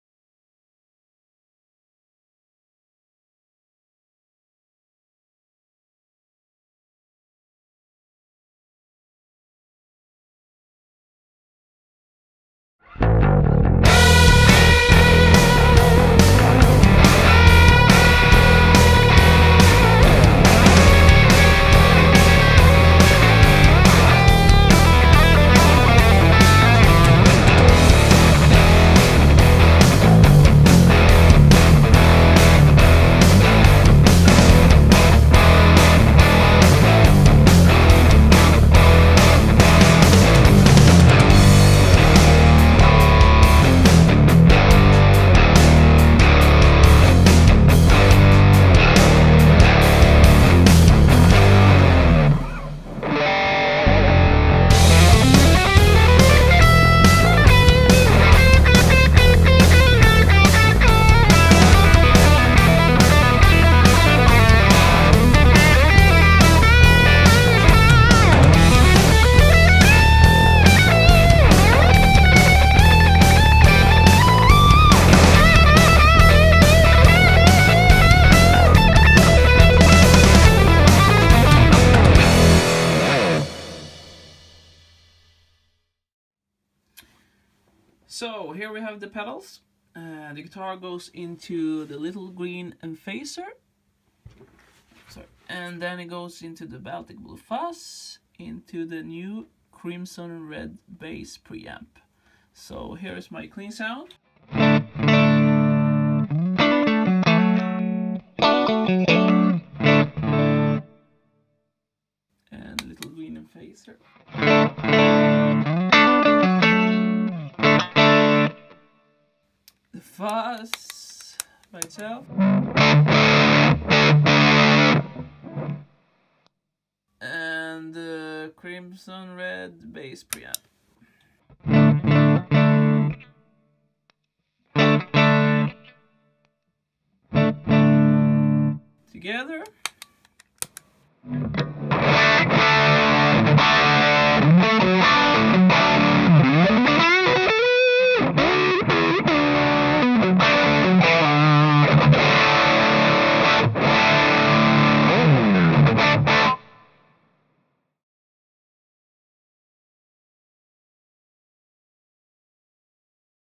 One Control stacked pedal video - Hans Johansson
One Control pedals stacks really well. Here is a great combination for a huge fuzz sound. One control pedals are designed by Björn Juhl (BJF)